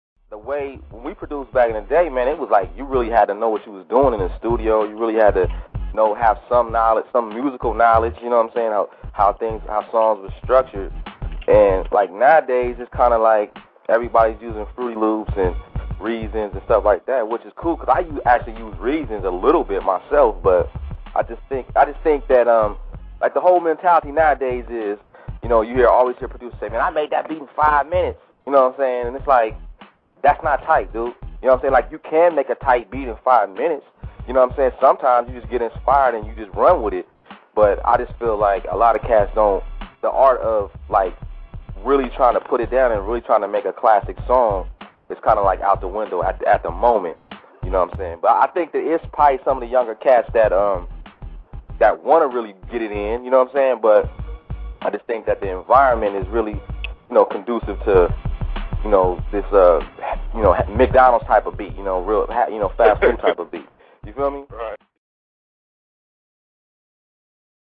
BLACK JACK SAYS PRODUCING BACK IN THE DAY WAS BETTER! YOU HAD TO KNOW WHAT YOU WERE DOING!
Black Jack let's it be know that young producers need to take more time in creating a track for artist.